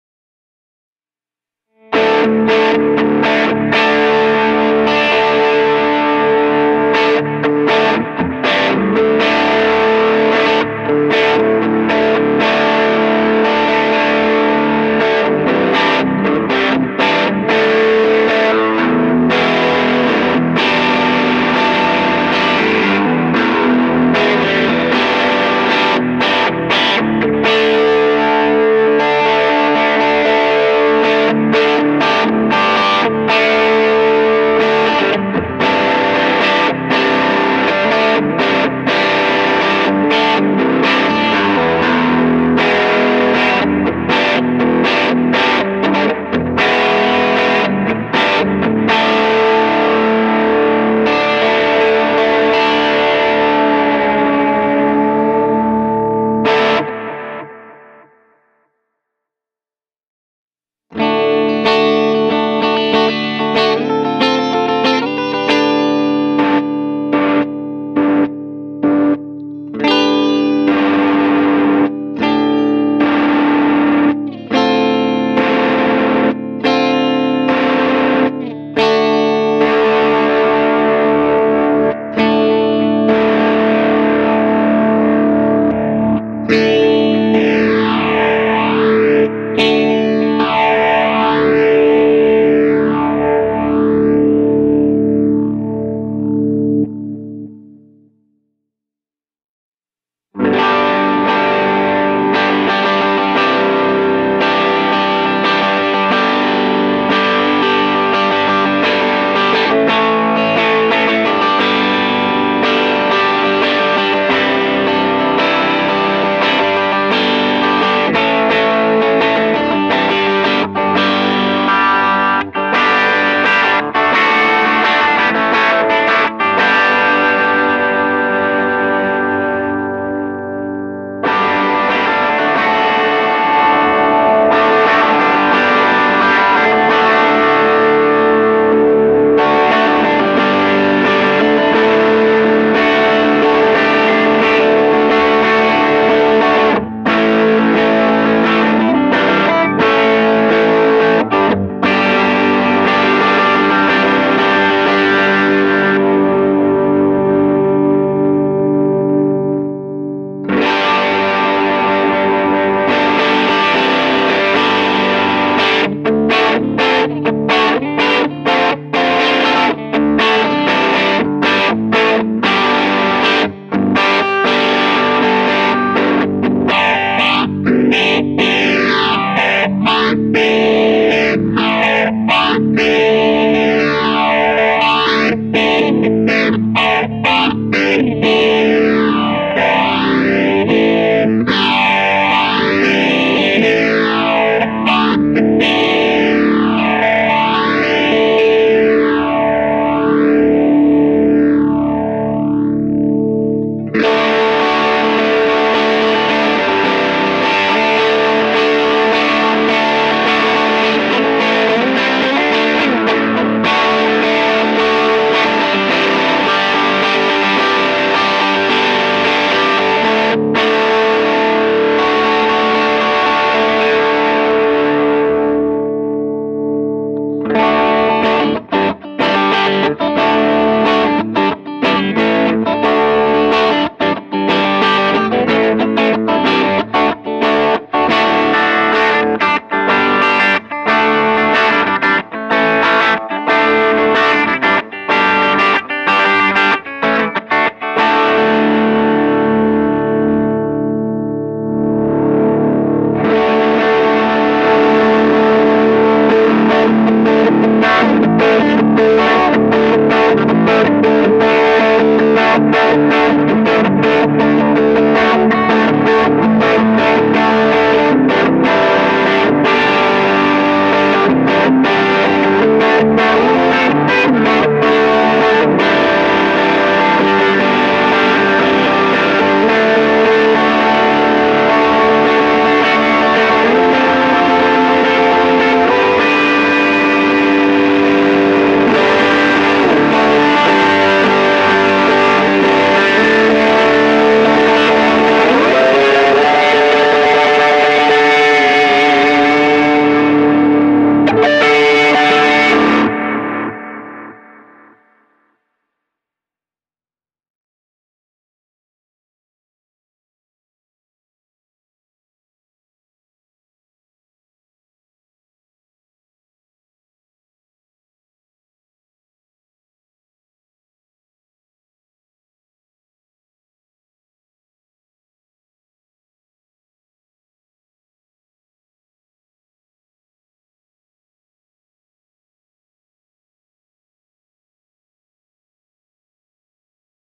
NativeAudio Kiaayo Overdrive
The NativeAudio Kiaayo Overdrive is based on the idea that less-is-more and provides a streamlined interface for an unlimited overdrive experience. Implementing a 3-knob layout, the Kiaayo offers an adaptable-gain configuration, distinctive pre-gain low-pass filter, and dynamic-asymmetrical clipping. The result is an amp-like overdrive that responds to the dynamics of your playing for sustained leads and articulate chording.

Current Pedalboard:

29 Pedals EUNA
Dawes Audio rehoused Boss DD-3 Digital Delay

Boss RC-10R Rhythm Loop Station